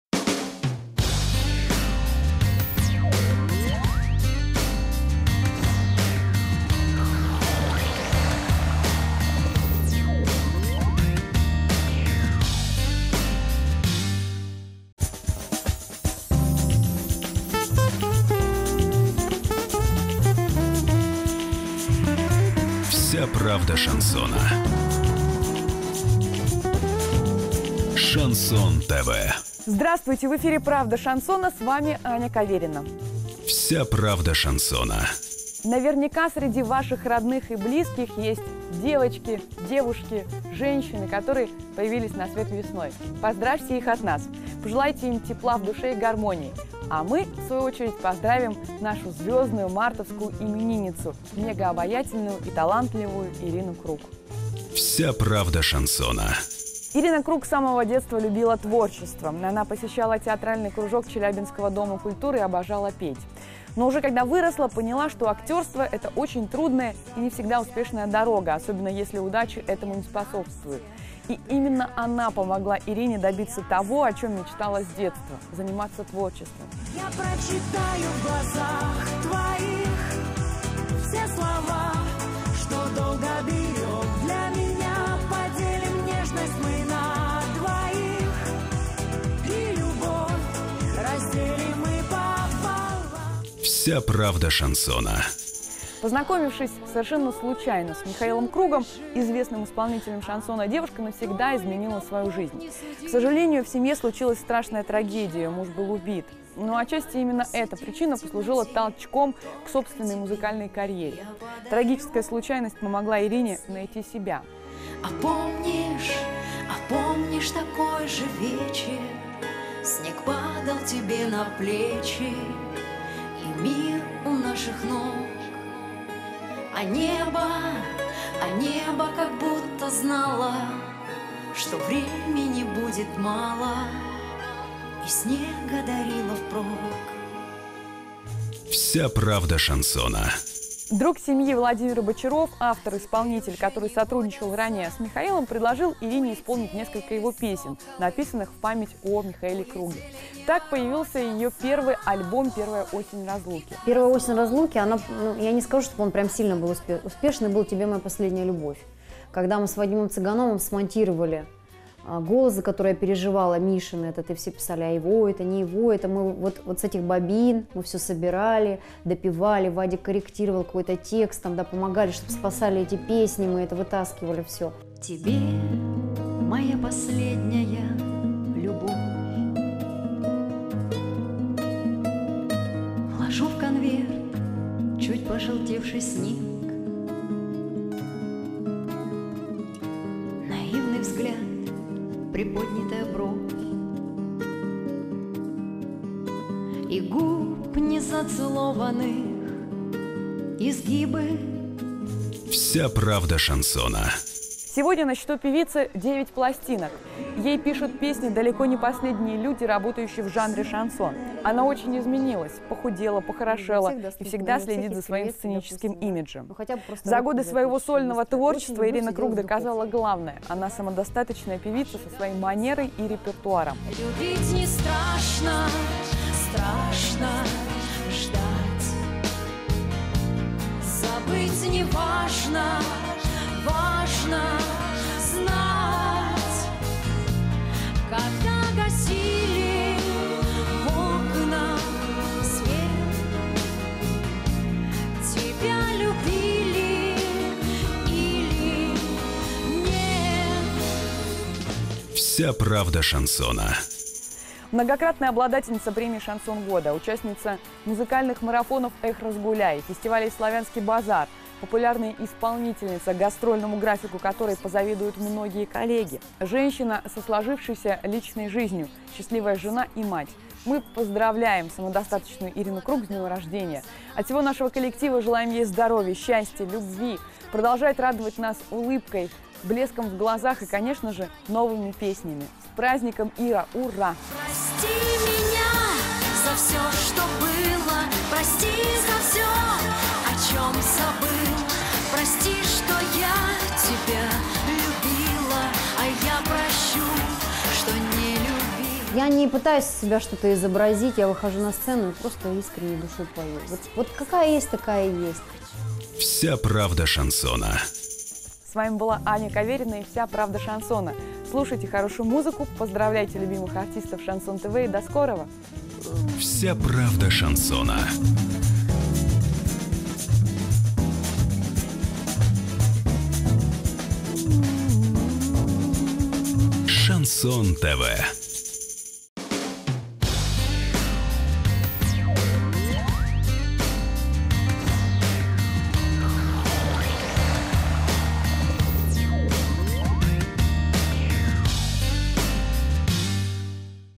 Ирина КРУГ, С ДНЕМ РОЖДЕНИЯ!
Шансон ТВ поздравляет Ирину КРУГ с днем рождения!

Когда поздравляешь женщину с днем рождения, главное – не называть ее возраст! Но это правило не касается Ирины КРУГ, ведь ей всегда 18 =)

Ее улыбка покорила миллионы мужских сердец, а проникновенные песни заставляют даже самых суровых людей стать мягче и окунуться в светлую грусть. Ирина КРУГ удивительным образом совмещает в себе красоту внешнюю и чистоту внутреннюю. Ее движения отличаются мягкостью и грациозностью. Любой, кто хоть немного пообщается с ней, на долгое время остается под впечатлением, его жизнь наполняется красками, а душа – светом и теплом.

29 марта принцесса Шансона отмечает свой день рождения! Весь коллектив телеканала Шансон ТВ от души поздравляет несравненную Ирину КРУГ с этим праздником! Мы желаем ей всегда оставаться такой же доброй, светлой и красивой!

Спасибо вам, Ирина, за все, что вы делаете.

Спасибо от лица всех любителей настоящей душевной музыки.